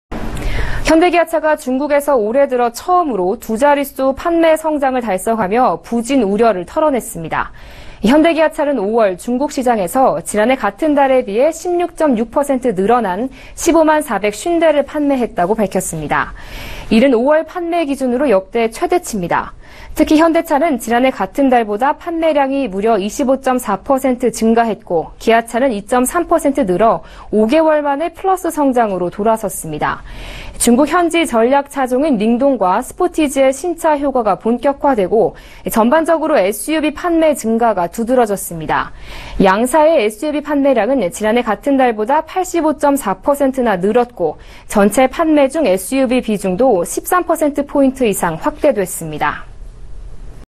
[서울경제TV] 현대기아 5월 中 판매 16.6%↑… 부진 우려 털어내
현대기아 총 15만대 판매… 역대 5월 최대치
현대차 전년동월比 25.4%↑… 기아차 5개월만 +성장
‘링동·스포티지’등 신차효과·SUV 고공행진
SUV판매량 전년동월比 85.4%… 비중 13%p↑

현대기아차가 중국에서 올해 들어 처음으로 두 자릿수 판매 성장을 달성하며, 부진 우려를 털어냈습니다.
현대기아차는 5월 중국 시장에서 지난해 같은 달에 비해 16.6% 늘어난 15만450대를 판매했다고 밝혔습니다.
이는 5월 판매 기준으로 역대 최대치입니다.
특히 현대차는 지난해 같은 달보다 판매량이 무려 25.4% 증가했고, 기아차는 2.3% 늘어 5개월 만에 플러스 성장으로 돌아섰습니다.
중국 현지 전략차종인 ‘링동’과 스포티지의 신차 효과가 본격화되고, 전반적으로 SUV 판매 증가가 두드러졌습니다.
양사의 SUV판매량은 지난해 같은달 보다 85.4%나 늘었고, 전체 판매 중 SUV 비중도 13%p 이상 확대됐습니다.